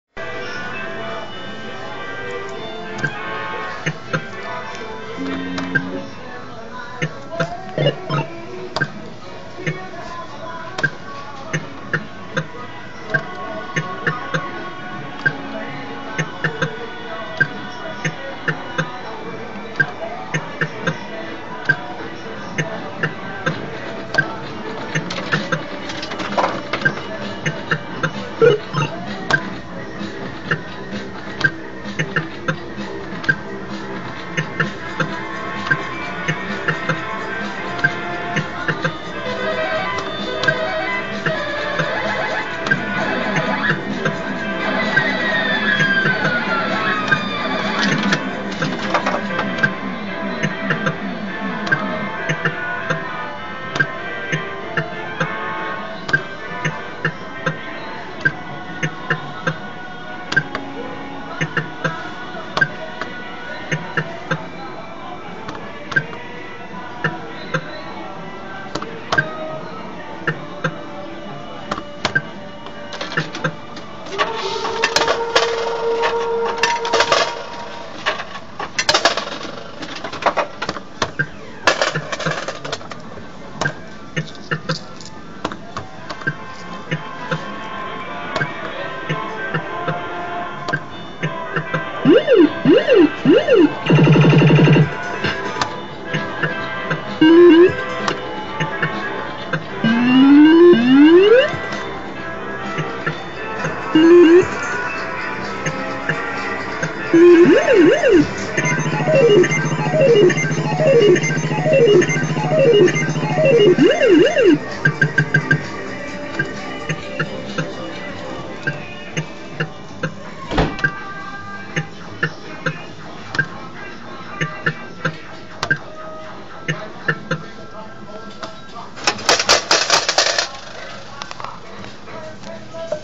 Red Gaming - Super Line Up 1
Another rebuild of a classic machine. What do I think? Complete rubbish! Exchanges Unlimited rebuild appears to be better.

Still, nice to see an old machine get a makeover.